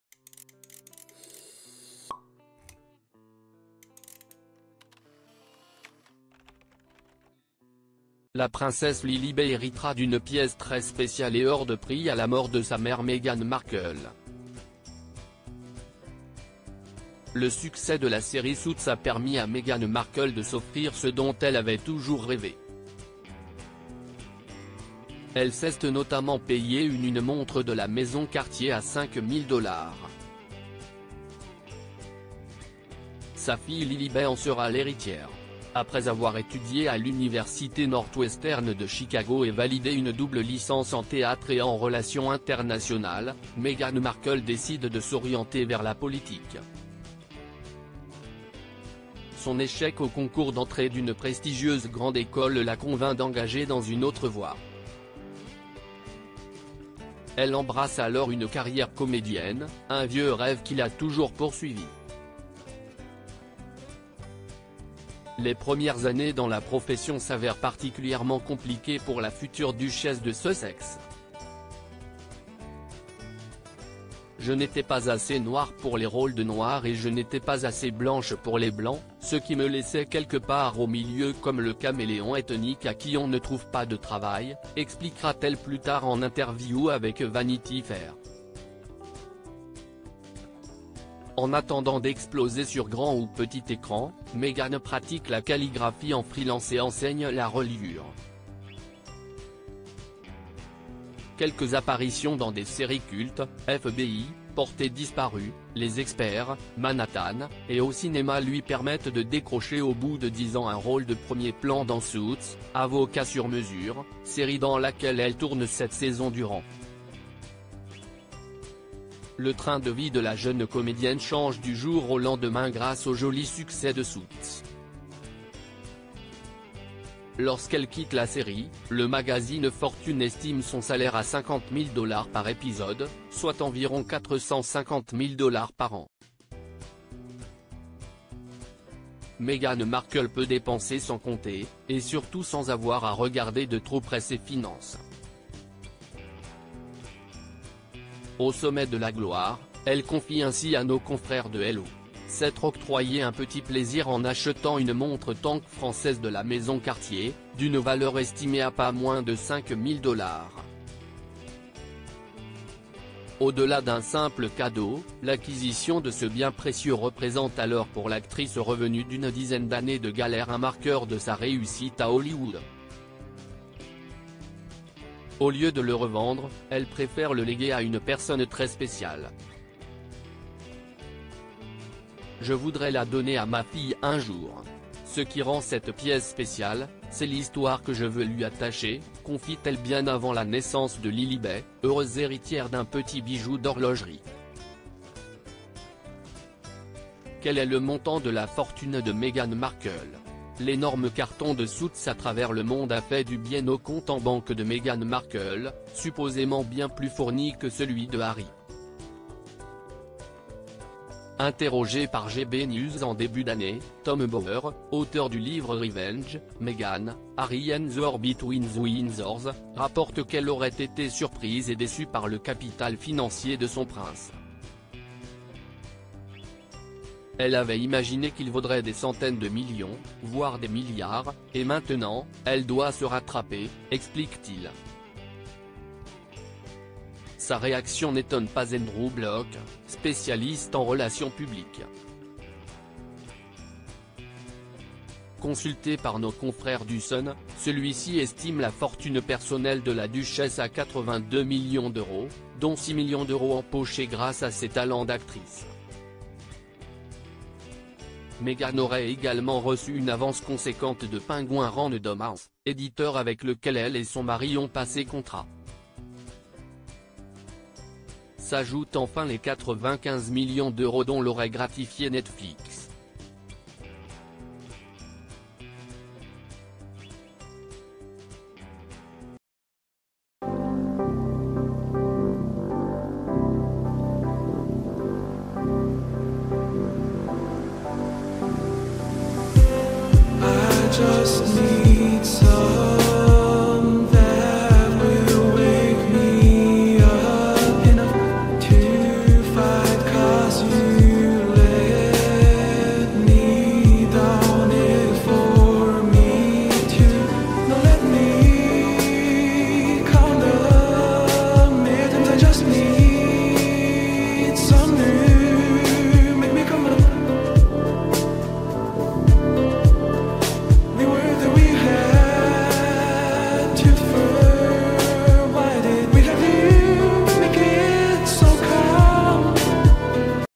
Meghan Markle ce cadeau hors de prix et très symbolique qu’elle avait prévu d’offrir à Lilibet ava
Meghan Markle : ce cadeau hors de prix et très symbolique qu’elle avait prévu d’offrir à Lilibet avant même qu’elle soit née